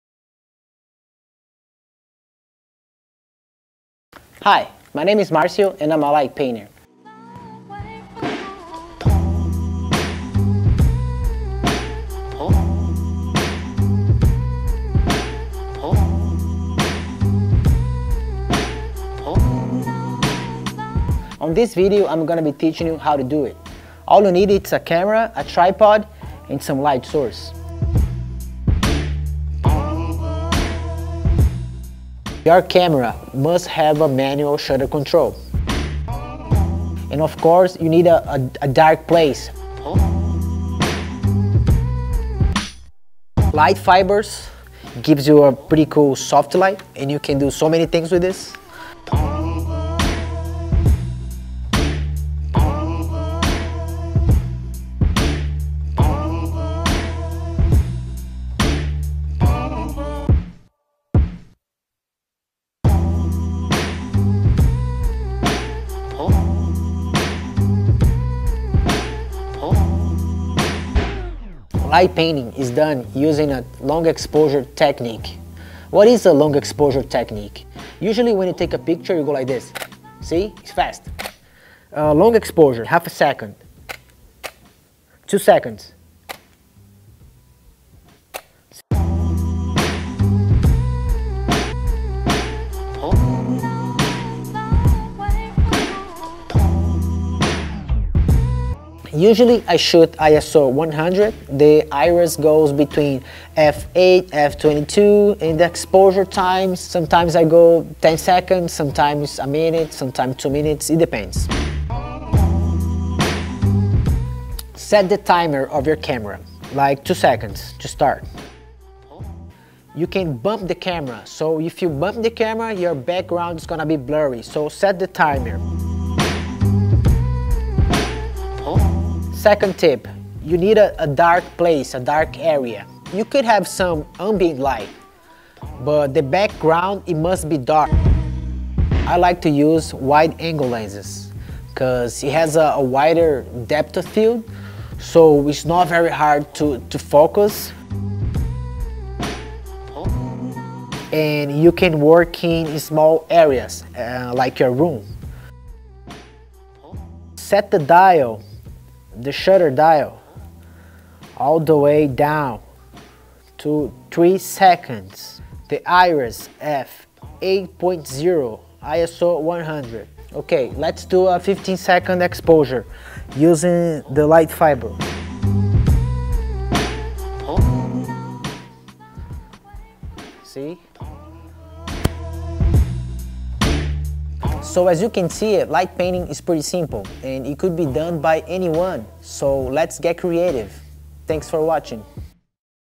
Light Painting Tutorial with Versalume Lighting Technology
Enjoy this Light Painting Tutorial using Versalume Lighting Technology. Light Painting is a long exposure photography technique that creates unique pieces of art.